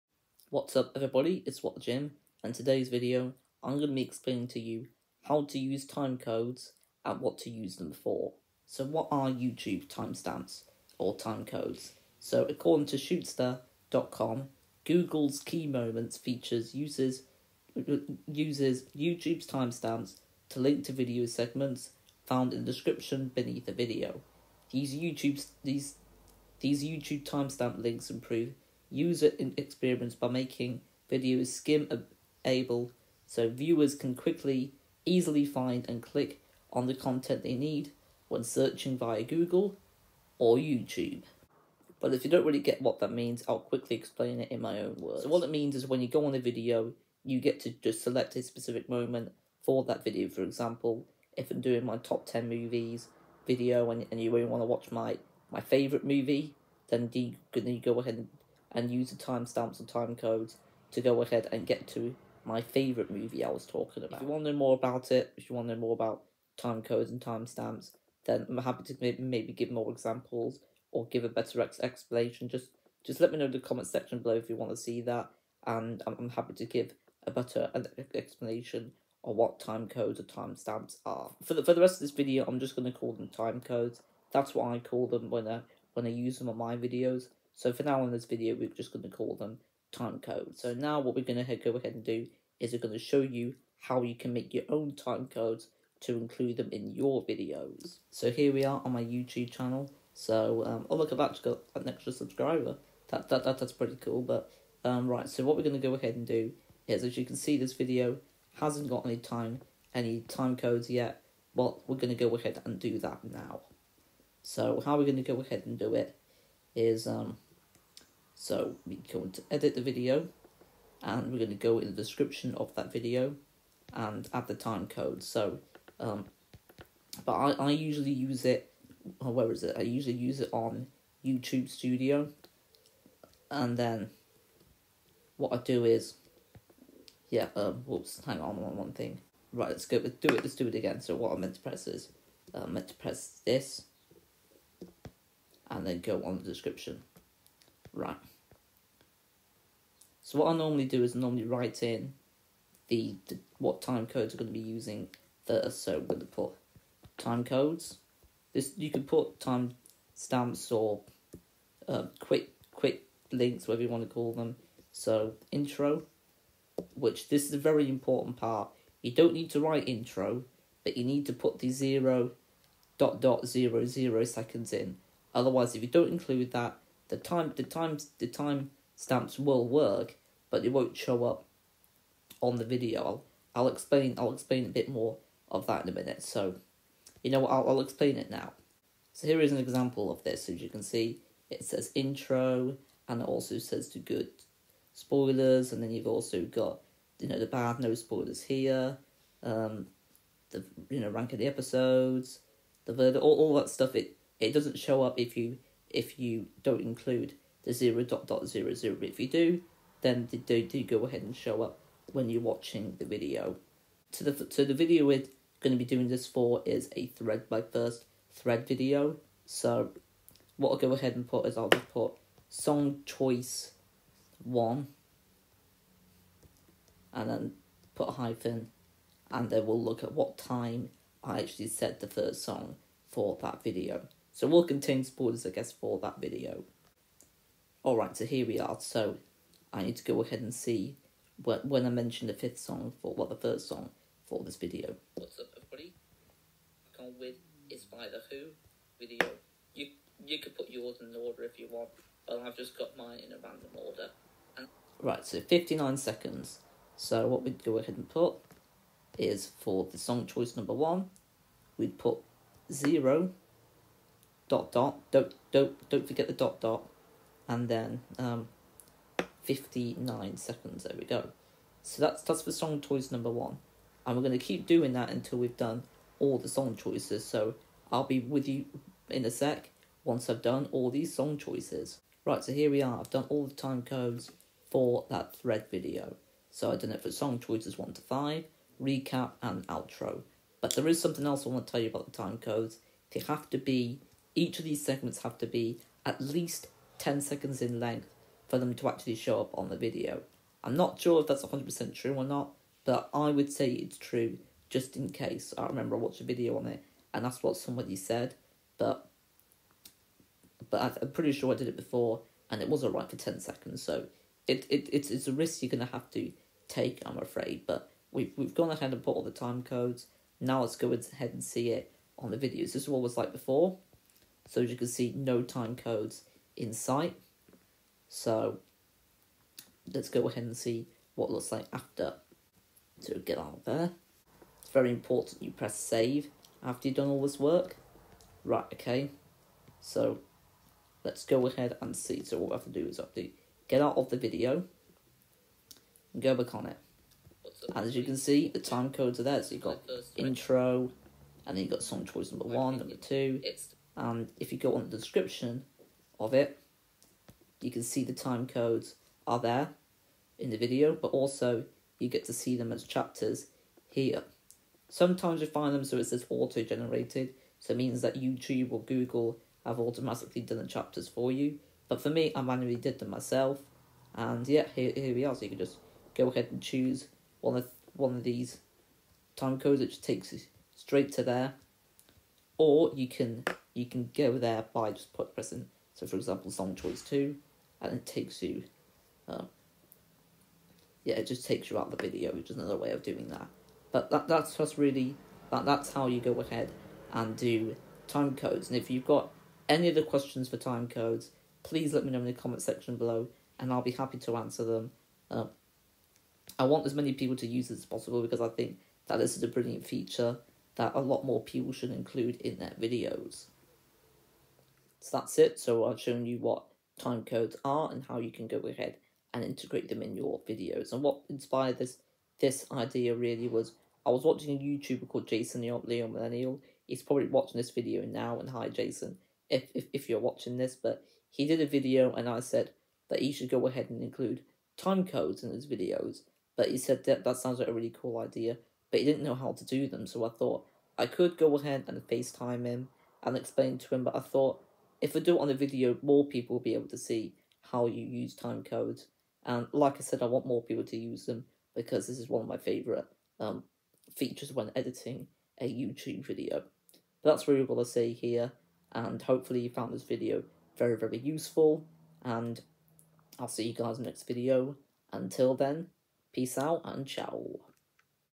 Time codes: What they are & how to use them!!!
Time codes:

Intro-0:00

What time codes are-0:08

How to create time codes-1:38

Outro-6:55